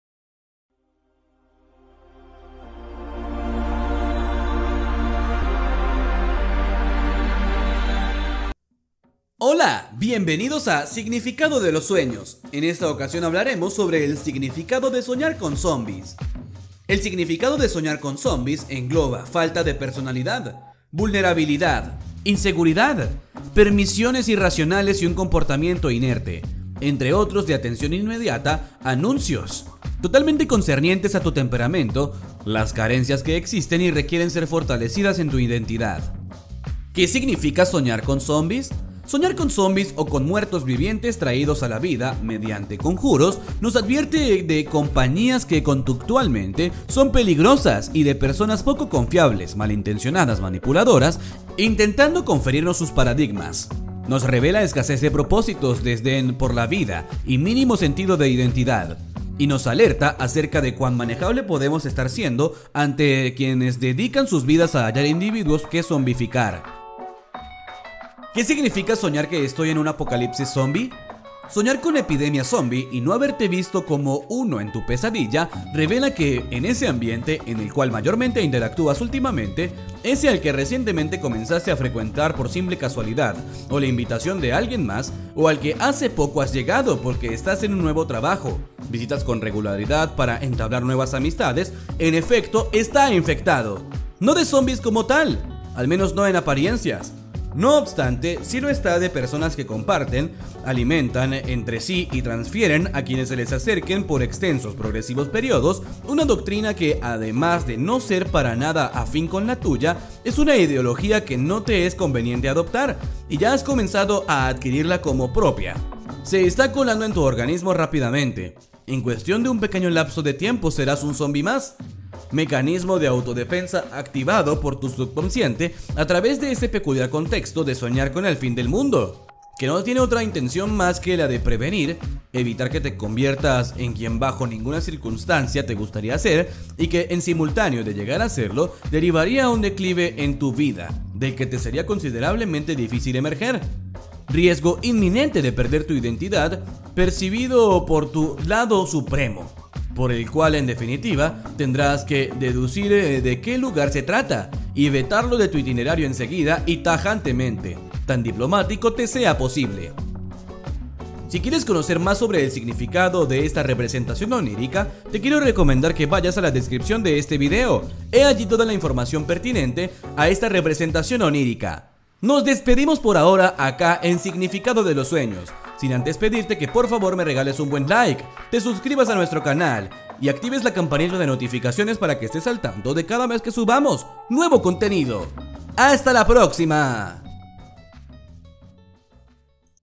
Significado de soñar con zombies | Significado de los sueños
¿Soñaste con zombies y querés interpretarlo?
Explicaremos el significado de soñar con zombies y :

    ¿Qué significa soñar con zombies?
    ¿Qué significa soñar que estoy en un apocalipsis zombie?
    ¿Qué significa soñar con zombies persiguiéndome?
    ¿Qué significa soñar con zombies y sobrevivir?
    ¿Qué significa soñar con ser un zombie?
    Otros significados de soñar con zombies

Deja un LIKE si te gusto el video y un comentario con tu sueño :)